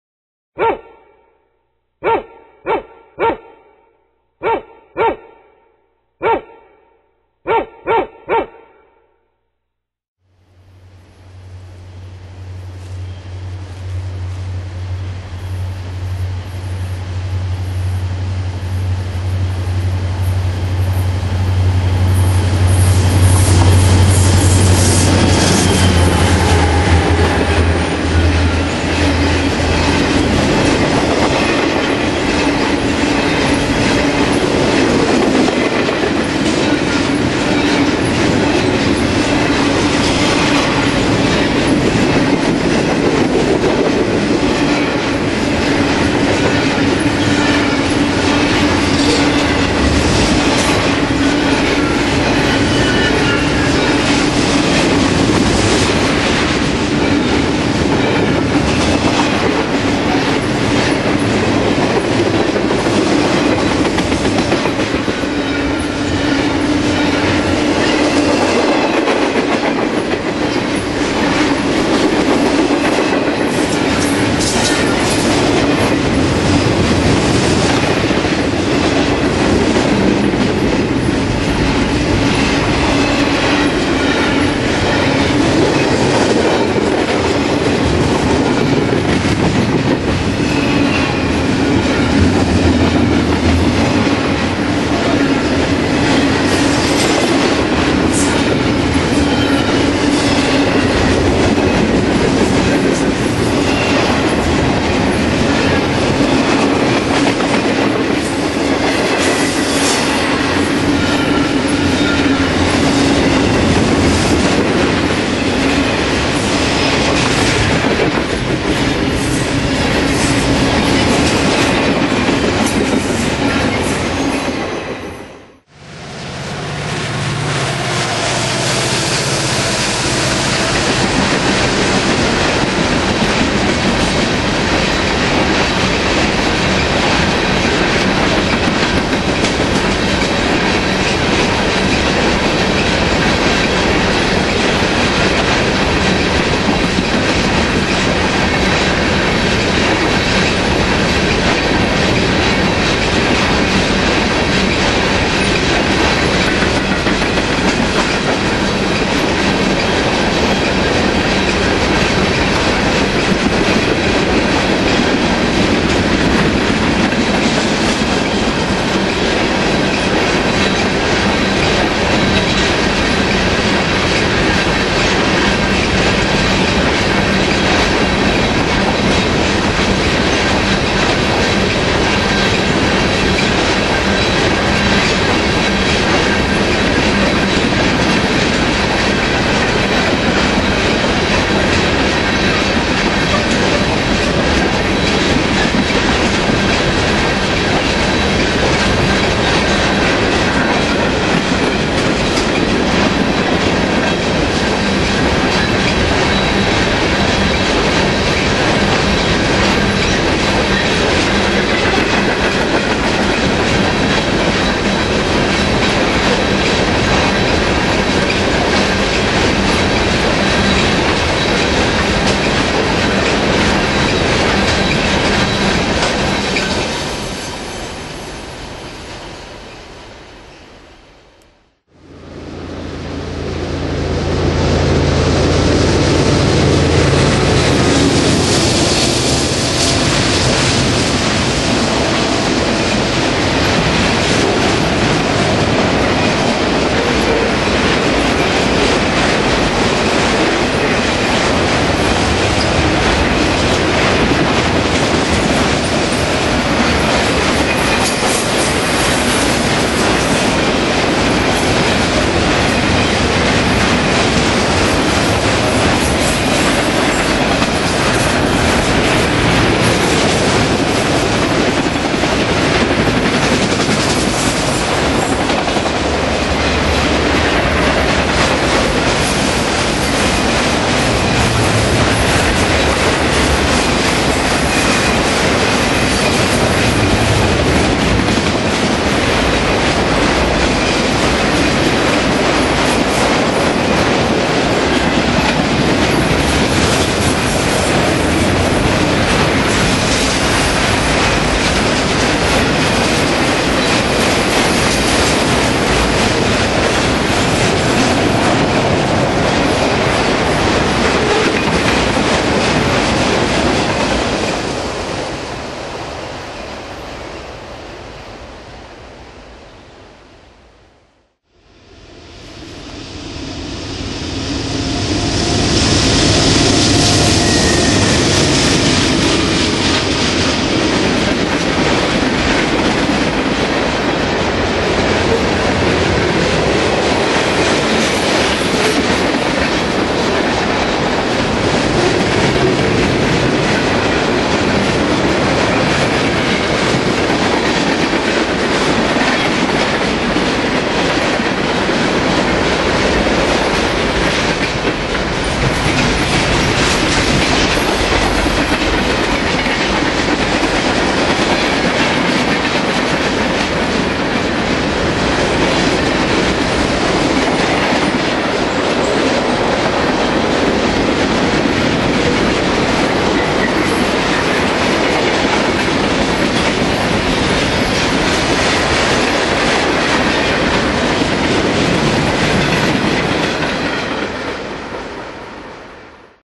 An afternoon at Maiden Rock
Trains by the river just north of Maiden Rock, Wi.